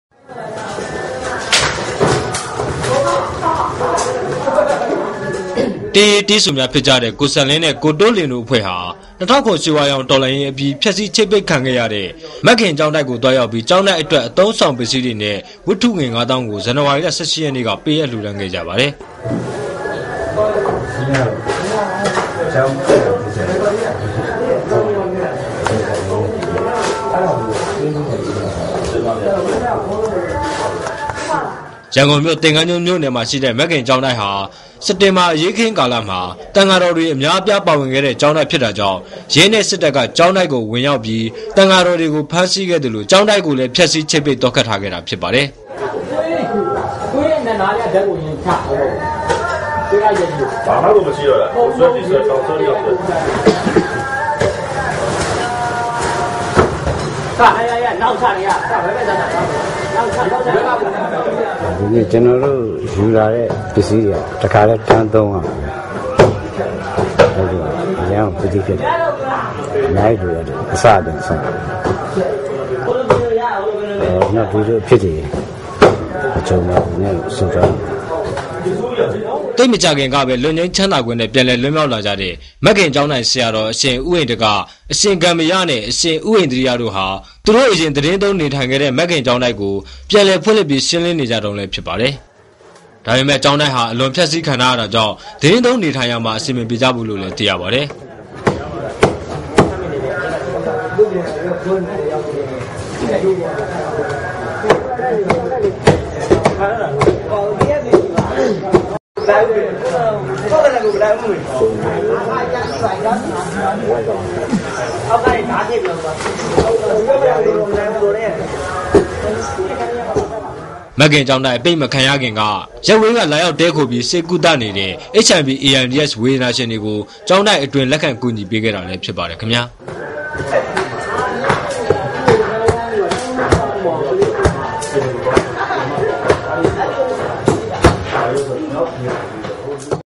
Artists San Linn and Toe Lwin donate to Maggin Monastery
ေတးေရး ေတးဆုိမ်ားျဖစ္ၾကတဲ့ ကုိစံလင္း န႔ဲ ကုိတုိးလြင္တုိ႔အဖြဲ႔ဟာ ၂၀၀၇ ေရႊ၀ါေရာင္ေတာ္လွန္ေရးအၿပီး ဖ်က္ဆီး ခ်ိတ္ပိတ္ခံခဲ့ရတဲ့ မဂၢင္ေက်ာင္းတုိက္ကုိ သြားေရာက္ၿပီး ေက်ာင္းတုိက္အတြက္ အသုံးအေဆာင္ ပစၥည္းေတြနဲ႔ ၀တၳဳေငြ ၅ ေသာင္းကုိ ဇန္န၀ါရီလ ၁၈ ရက္ေန႔က ေပးအပ္လွဴဒါန္းခဲ့ၾကပါတယ္။
လာလွဴတဲ့ပစၥည္းေတြကုိ ေက်ာင္းတုိက္မွာ ခ်က္ျခင္းအသုံးအျပဳႏုိင္တဲ့အတြက္ ပိတိျဖစ္ရတယ္လုိ႔  ေတးဆုိ ကုိစံလင္းက ဆုိပါတယ္။ရန္ကုန္ၿမိဳ႕ သကၤန္းကြ်န္းၿမိဳ႕နယ္မွာရွိတဲ့ မဂၢင္ေက်ာင္းတုိက္ဟာ စက္တင္ဘာအေရးအခင္း ကာလမွာ သံဃာေတာ္ေတြအမ်ားအျပားပါ၀င္ခဲ့တဲ့ ေက်ာင္းတုိက္ျဖစ္တာေၾကာင့္ ရဲနဲ႔ စစ္တပ္က ေက်ာင္းတုိက္ကုိ ၀င္ေရာက္ၿပီး သံဃာေတာ္ေတြကုိ ဖမ္းဆီးခဲ့သလုိ ေက်ာင္းတုိက္ကုိလည္း ျဖတ္ဆီး ခ်ိပ္ပိတ္ ေသာ့ခတ္ထားခဲ့တာျဖစ္ပါတယ္။